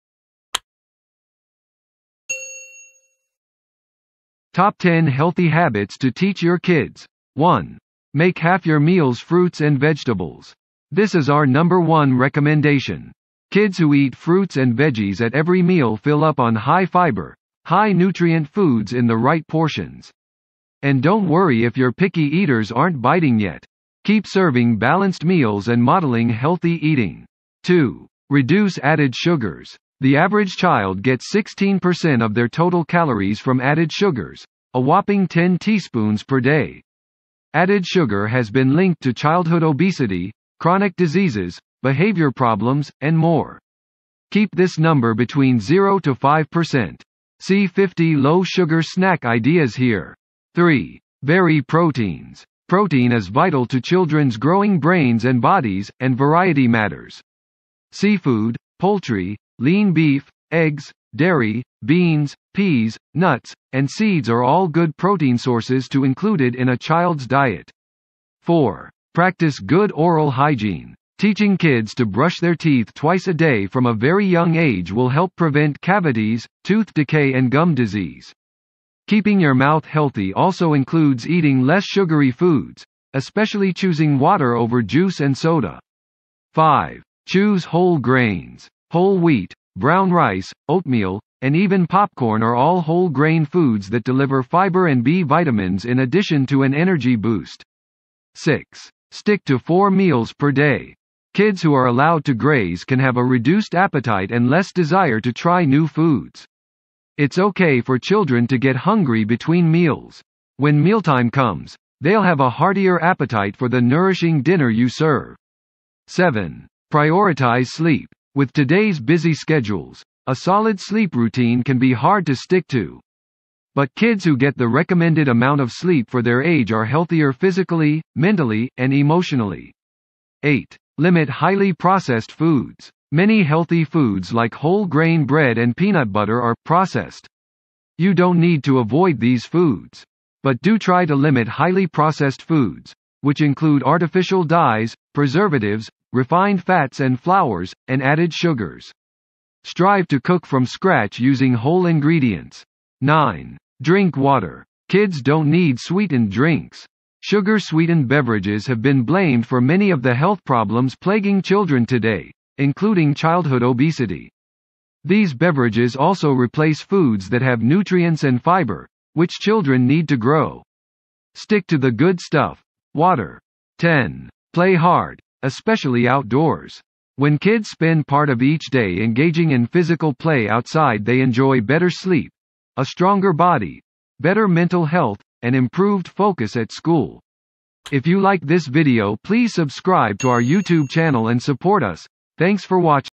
10 Healthy Habits to teach your kids | Top 10 | First cry
Welcome to 5.Min.Tube

In today's Tube - Every family benefits from adopting healthy habits, together! See our top 10 healthy habits for kids to find out which ones you’ve already achieved, and which ones to adopt as your next goal on your path to a healthier family.

Everyone wants their kids to be healthy and happy. But sometimes that end goal can feel overwhelming!

So we’re breaking down that big goal into ten manageable habits that your family can focus on to help you raise healthy kids.

In case TEN things sounds like a lot to remember, here’s more good news. You’re probably already implementing a few of these habits! Take a look at the 10 most important healthy habits for kids: